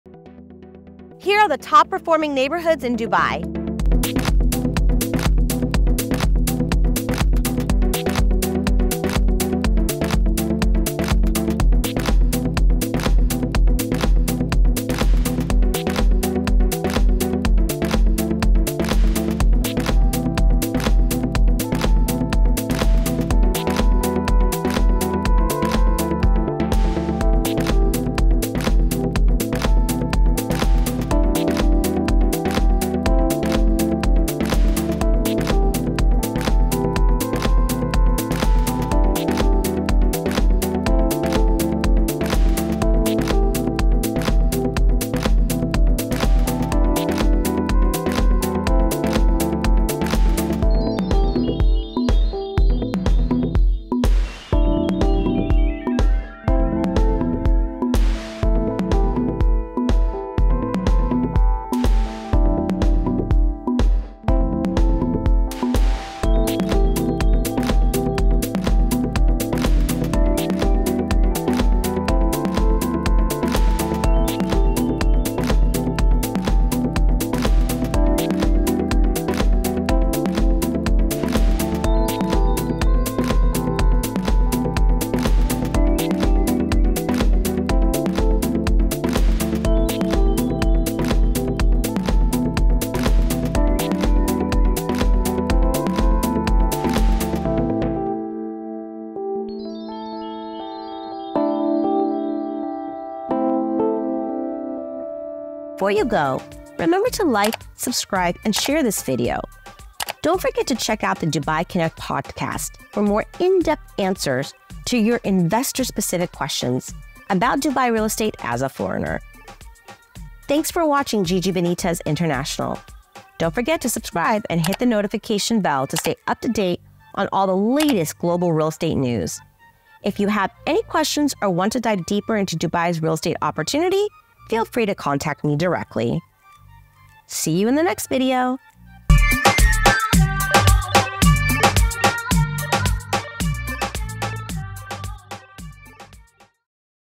I Found Top 5 Highest Performing Areas in Dubai Real Estate | Dubai Investment
Curious which areas in Dubai real estate are performing the best? This video presents the Top 5 Highest Performing Areas in Dubai Real Estate based on the most recent data and statistics.

To get more information about Dubai Real Estate contact in WhatsApp

We’ll show you the numbers, including transaction amounts, dollar figures, and more, for each neighborhood. These statistics highlight why these areas are topping the charts for investors:

1. Jumeirah Village Circle: Leading with strong rental yields and great value.
2. Dubai South: Positioned as the ‘New Dubai, with impressive growth thanks to its strategic location near Al Maktoum Airport and Expo City.
3. Business Bay: Central location driving high demand for luxury living and commercial spaces.
4. Wadi Al-Saffa: A neighborhood on the rise with a mix of luxury villas and apartments, gaining attention for its growth potential.
5. Dubai Hills Estate: A green community offering significant capital appreciation, making it one of the most desirable areas for investors.

As Dubai’s real estate market is incredibly dynamic, staying updated with the latest trends is key. Reach out to me so my team and I can guide you through the Top 5 Upcoming Off-Plan Project Investments in Dubai real estate and help secure your next big opportunity.

🎙 Podcast [The Dubai Connect Podcast™] httpsyoutube.complaylistlist=PLbj2rl9Iddu169OPoCPmQ9ysStcpbok8T&si=uZERZNCRGqDTtZ4y